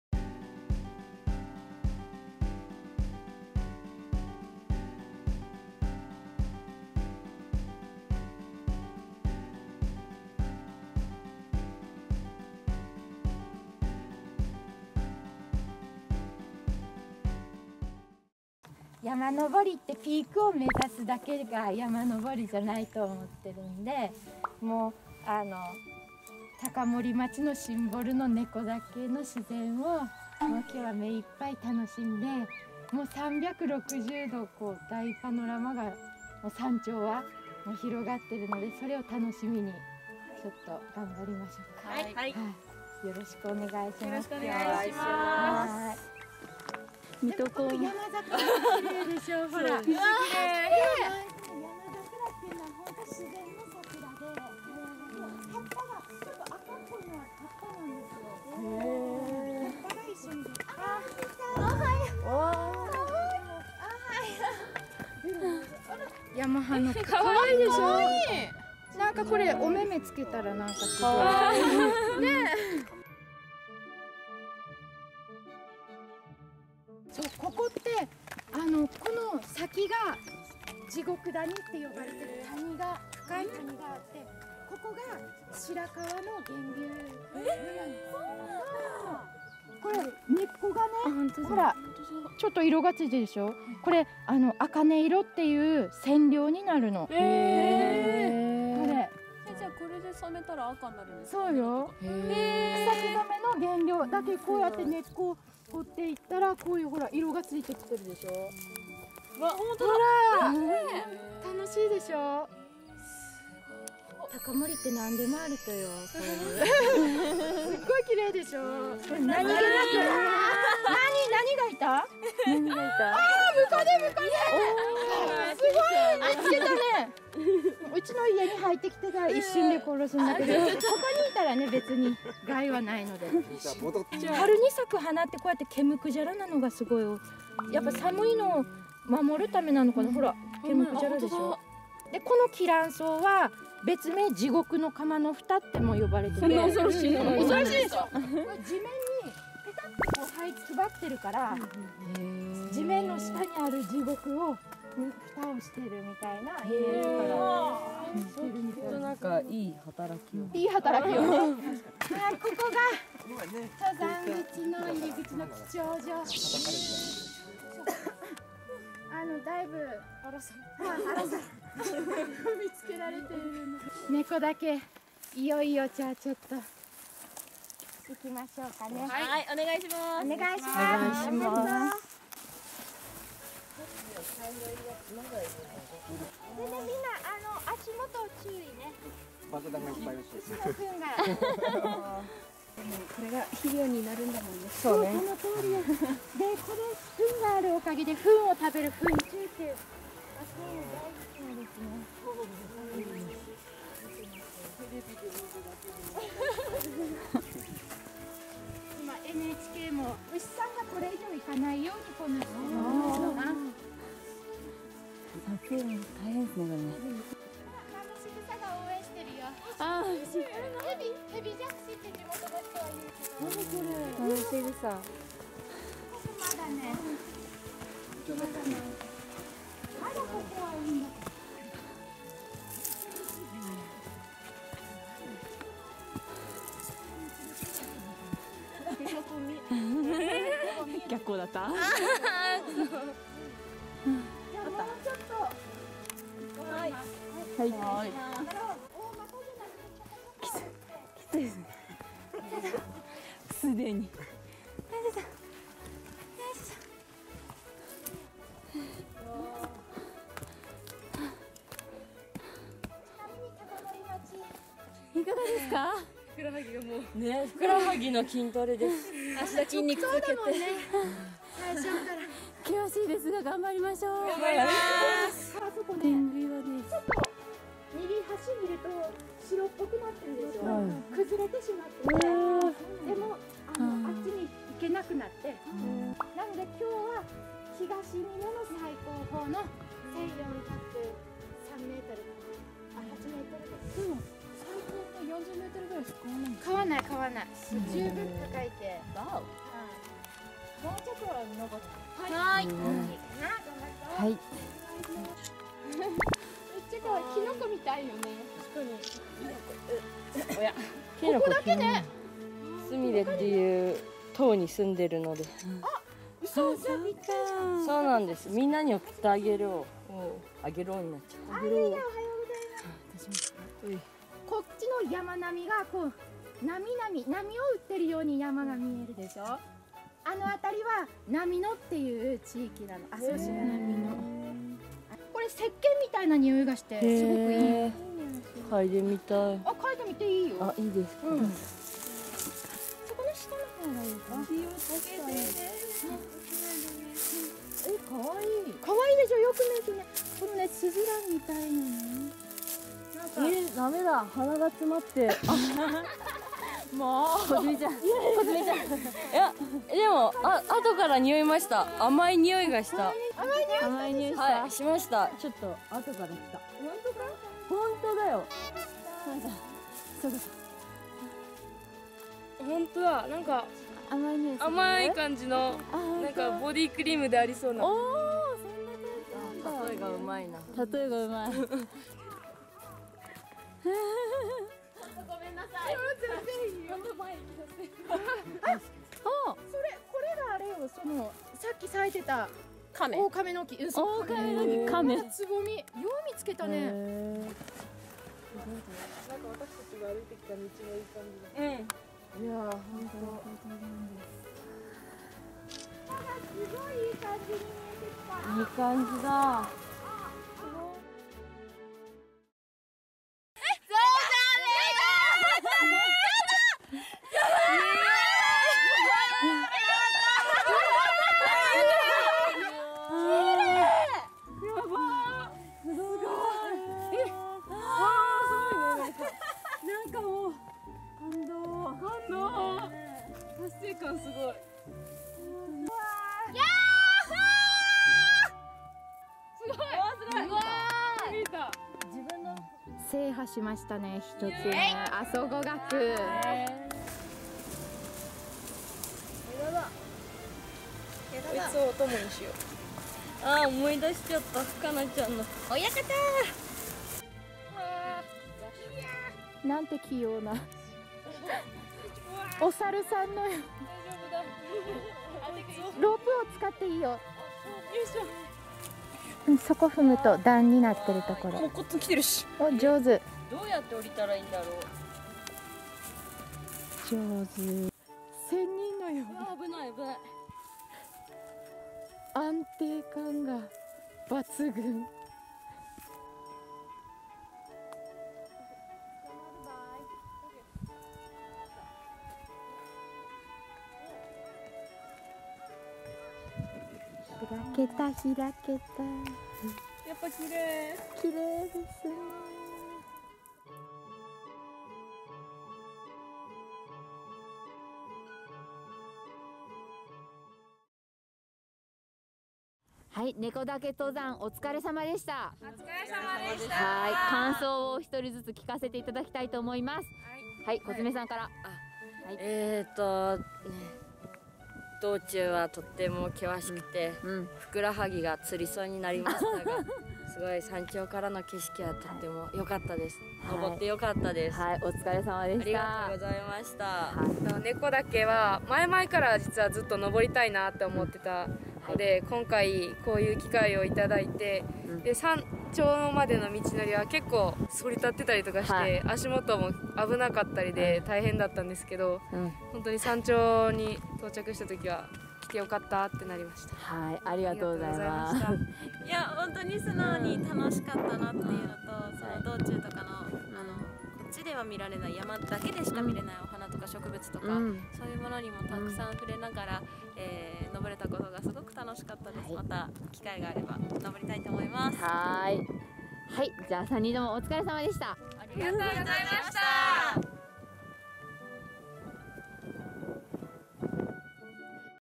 096k観光なんでも探検隊 根子岳登山
096k熊本歌劇団が根子岳登山を体験しました。

メンバー　096k熊本歌劇団
　　　　　鬼山　花菜さん
　　　　　窪田　莉緒さん
　　　　　福田　こつめさん

撮影：荒井沙織（政策推進課）編集：松尾日香里（政策推進課）地域おこし協力隊